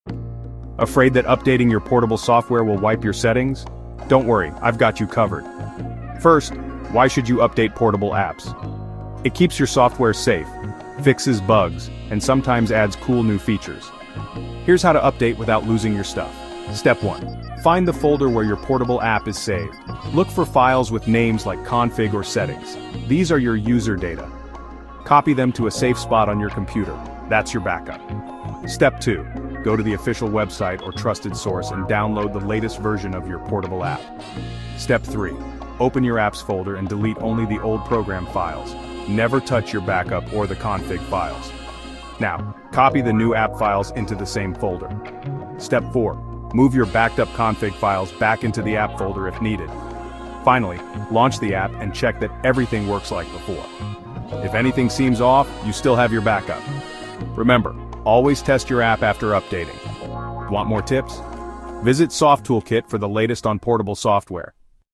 Cara update software portable tanpa hilang data!
Update software portable tanpa ribet dan tanpa kehilangan data penting!
Dalam video singkat ini, kamu akan belajar langkah-langkah aman untuk memperbarui aplikasi portable tanpa harus kehilangan konfigurasi atau file pentingmu. Cocok untuk pengguna software portable di flashdisk maupun laptop!

💡 Tips ini berlaku untuk software seperti:

OBS Portable
VLC Portable
Chrome Portable
dan lainnya!